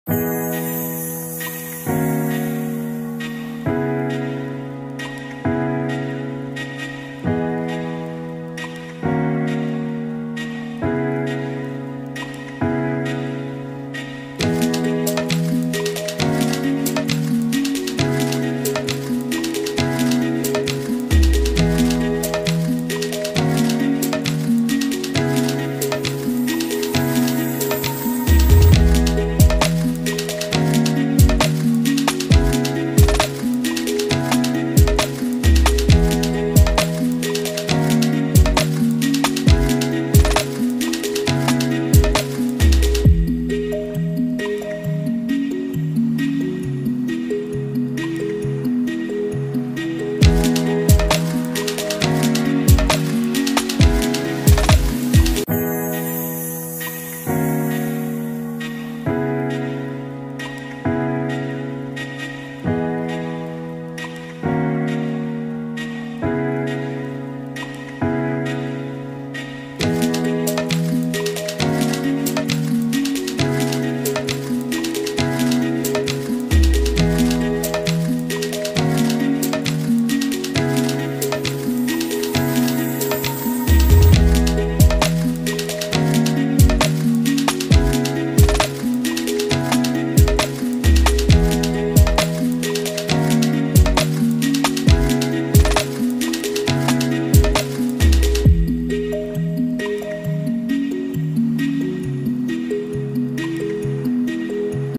小珍奶《正面香汗淋漓滴慢動作~直接反光很多汁的劉基鴻應援~果然可以代言諾貝爾(眼科)和平獎~》20240830海灘之約 天母棒球場 味全 dragon beauties
音樂家: Jeff Kaale